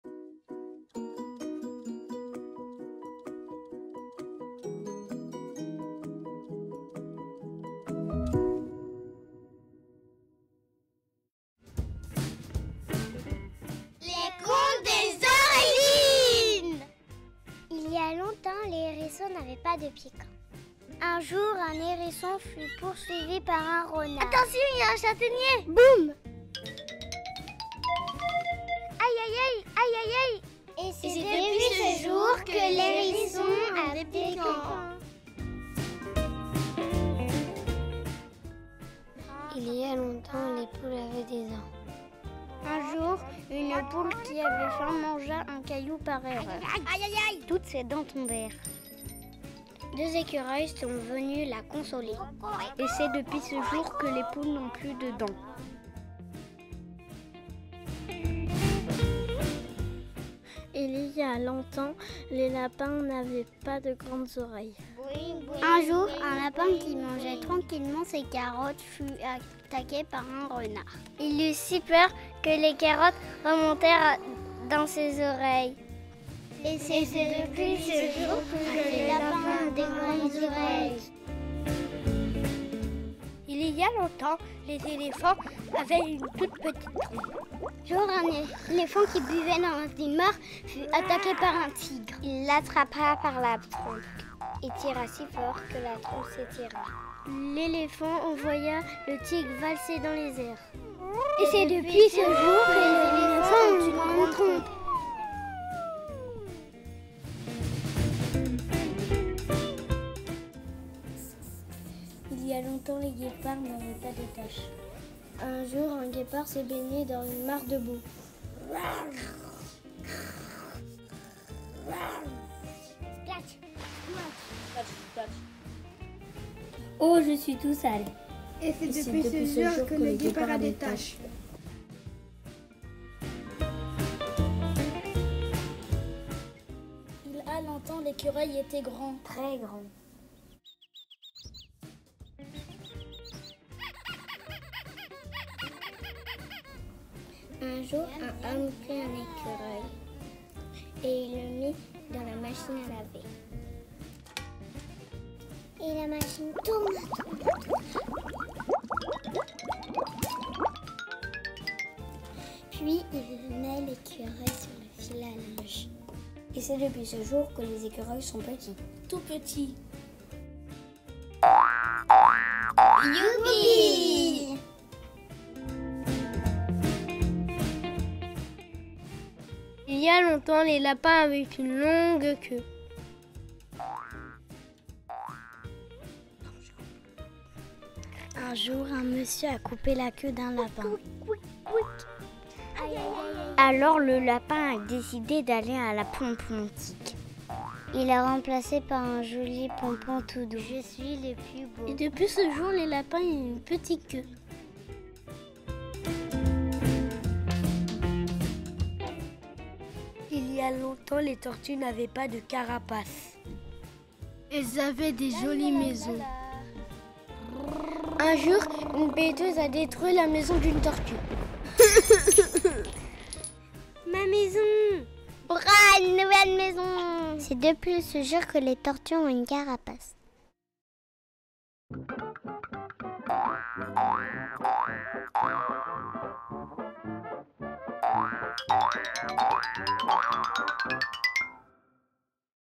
LES CONTES DES ORIGINES - stopmotion - CP- ecole Marie Curie - st Sebastien sur Loire - iKi iKi
Courts métrages inspirés des contes des origines réalisés en STOP MOTION par les élèves de CP de l’école élémentaire Marie Curie de st Sebastien sur Loire en partenariat avec l’association iKi iKi.
Juin 2021